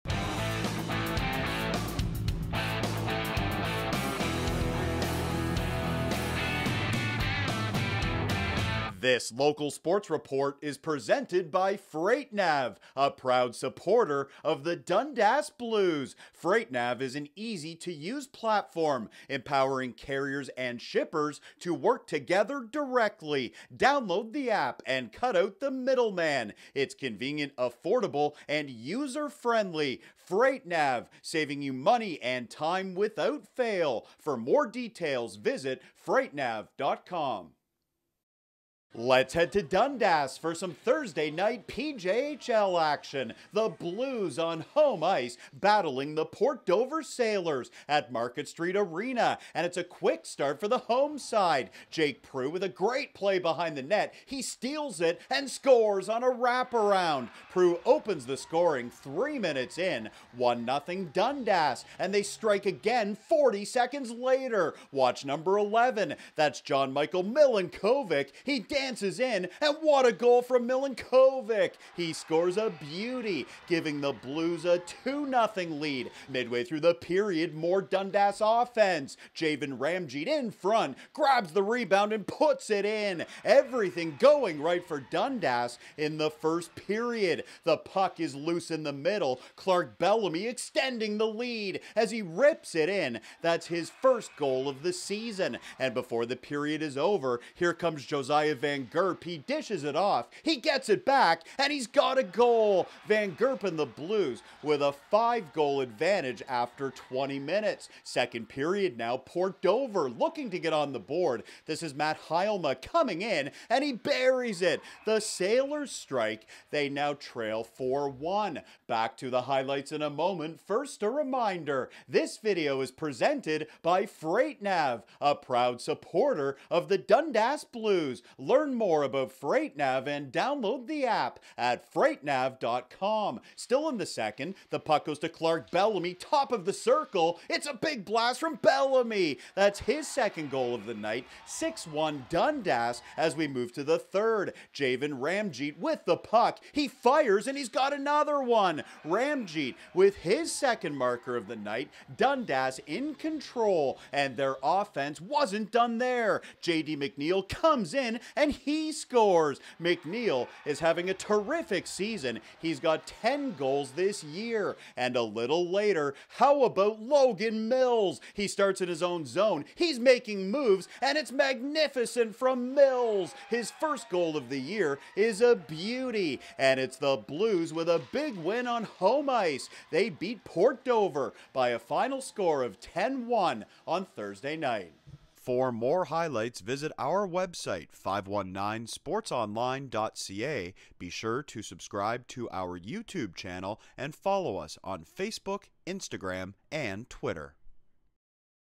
PJHL - Port Dover Sailors vs Dundas Blues
Highlights from Thursday November 14th between the Sailors and Blues at Market Street Arena.

This video is presented by Freight Nav, an easy to use platform empowering carriers and shippers to work together directly.